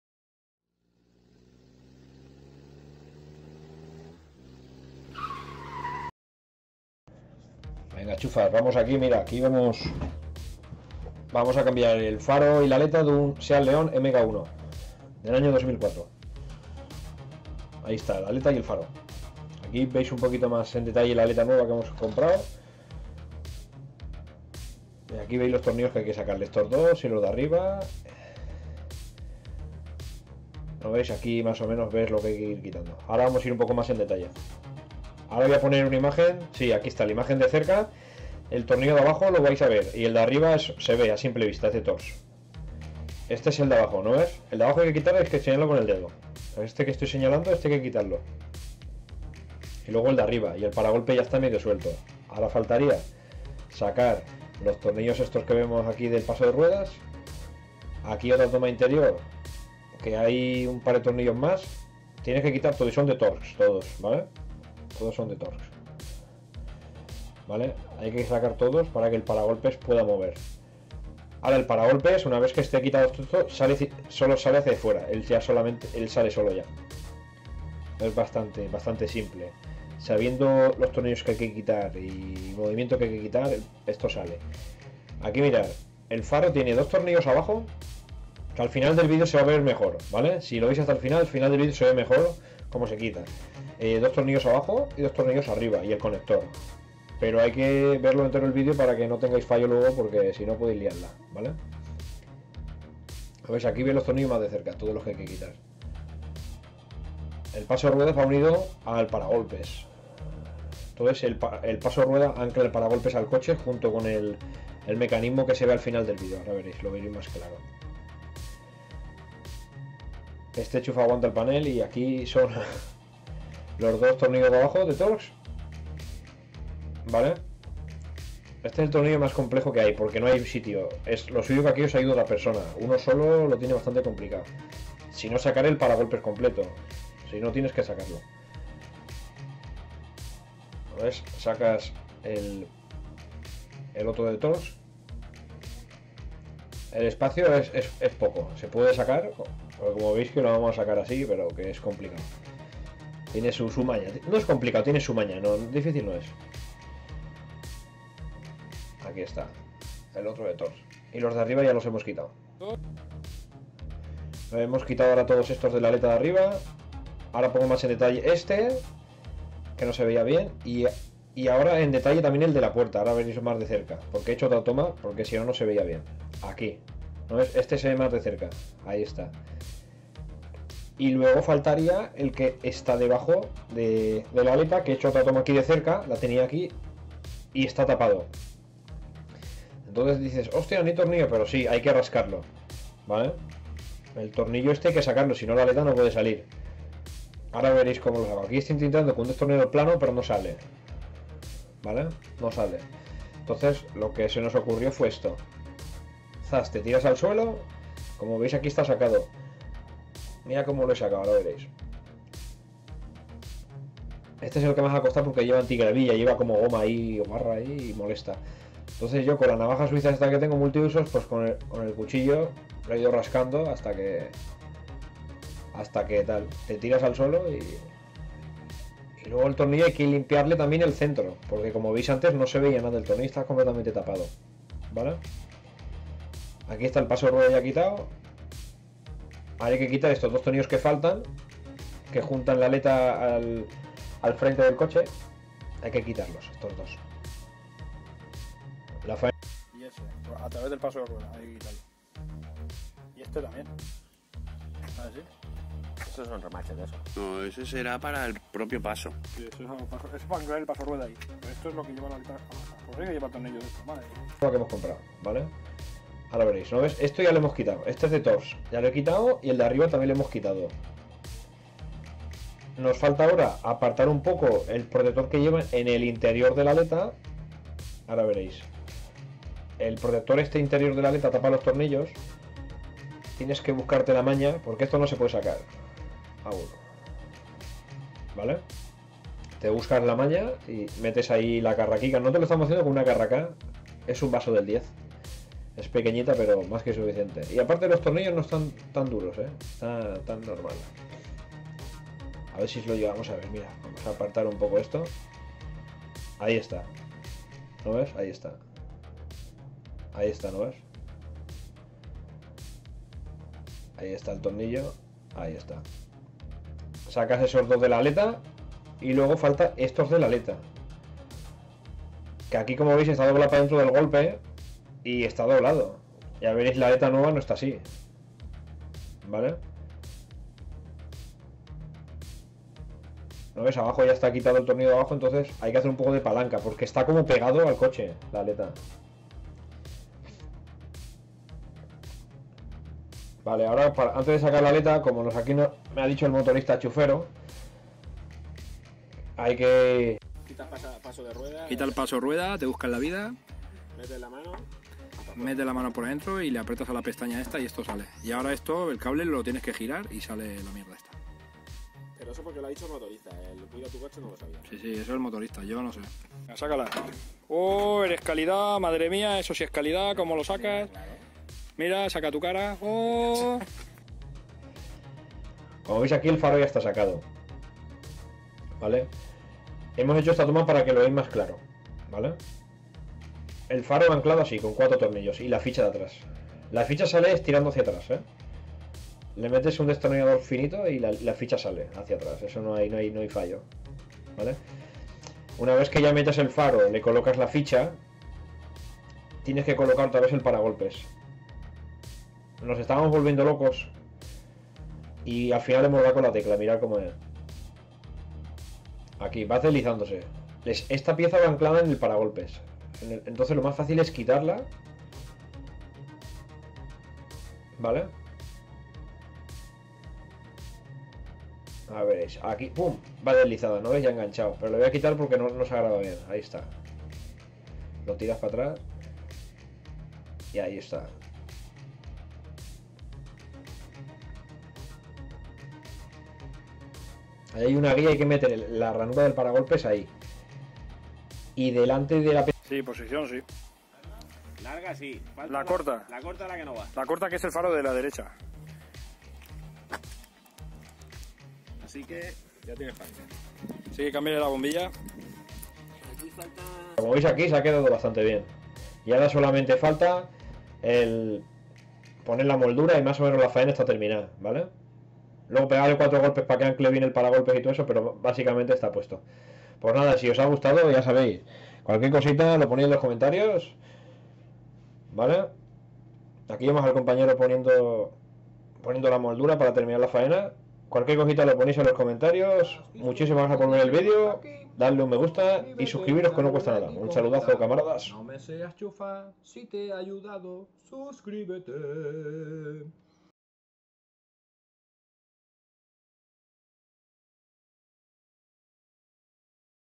Seat Leon MK1 - Cambiar aleta y faro delantero
hey camaradas , aqui ponemos como reparar o mejor dicho cambiar la aleta y el faro delantaro , a un Seat Leon , tambien serviria para un seat toledo , ya que el morro es igual , saludazos , y no me ayudado un amigo que tiene un canal de MOTOS meteis y no me seais chufas , gracias por verlo y hasta el siguiente video